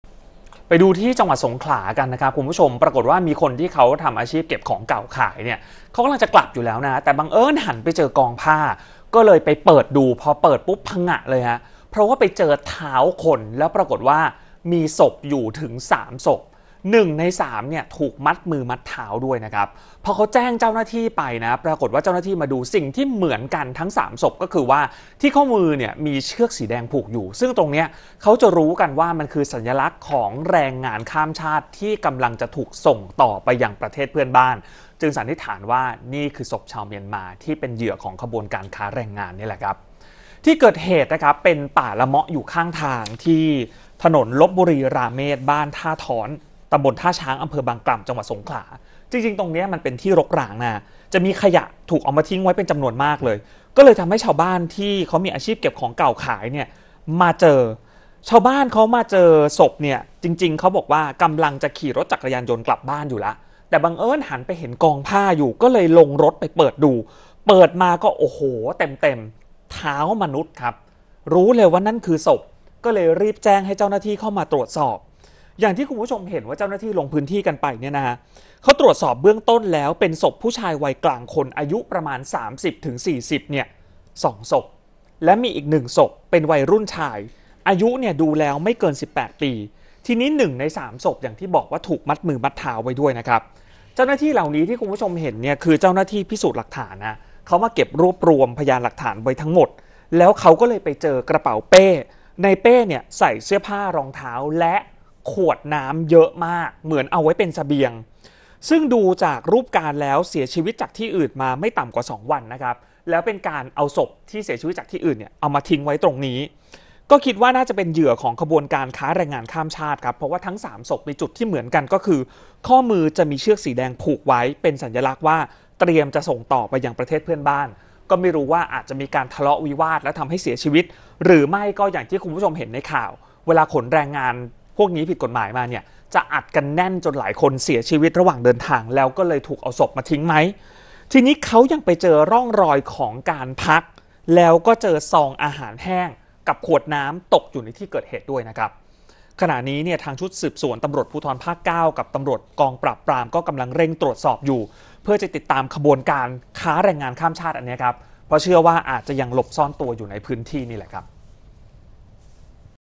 เจอศพชาวเมียนมา 3 คน ถูกทิ้งในป่าละเมาะ ข่าวค่ำ วันที่ 15 มีนาคม 2566
ที่จังหวัดสงขลา มีคนเก็บของเก่า ไปเจอศพชายชาวเมียนมา 3 คน ถูกทิ้งในป่าละเมาะ คาดว่า เป็นเหยื่อขบวนการค้าแรงงานข้ามชาติ เตรียมส่งไปยังประเทศเพื่อนบ้าน